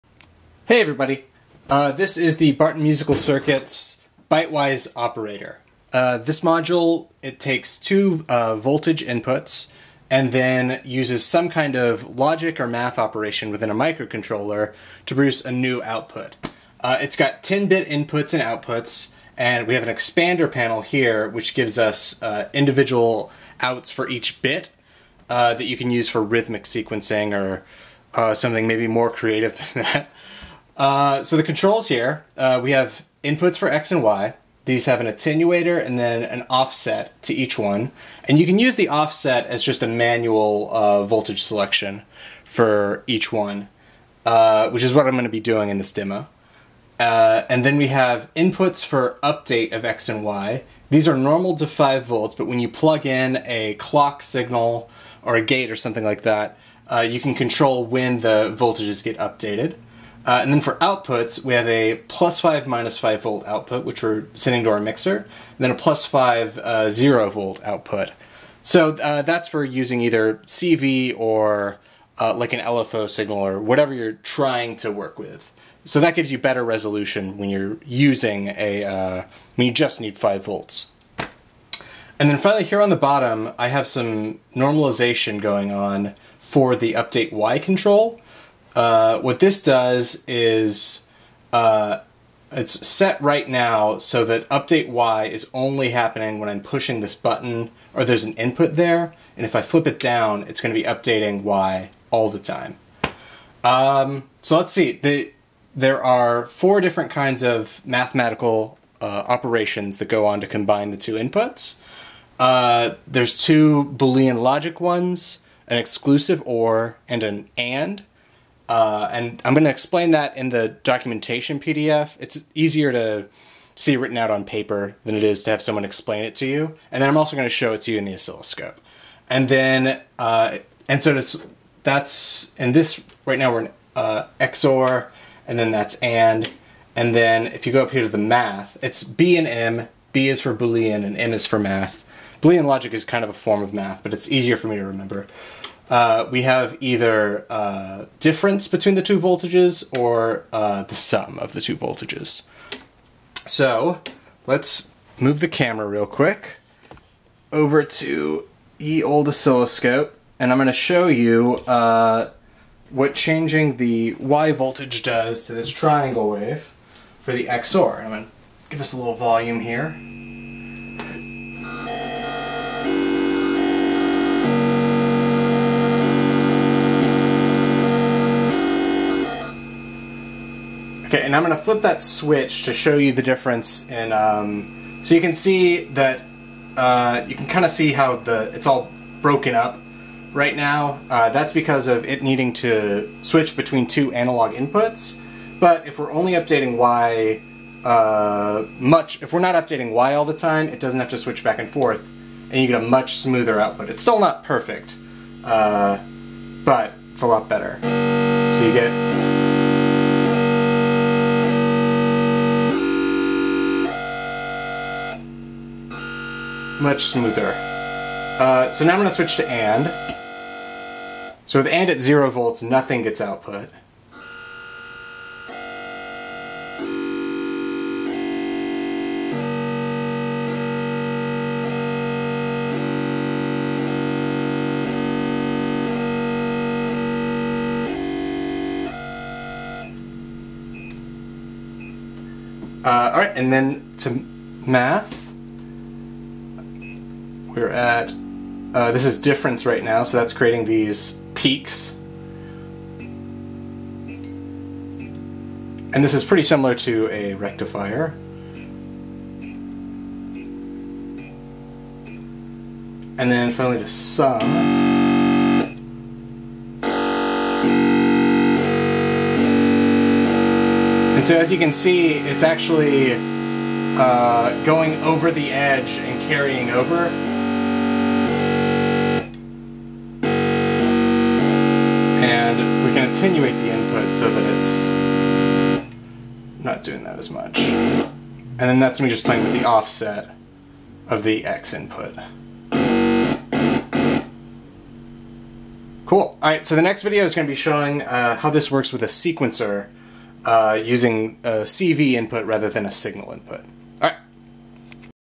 Bytewise Operator Demo 1 Overview/Waveshaping
This is a demo of a new synthesizer module I've designed.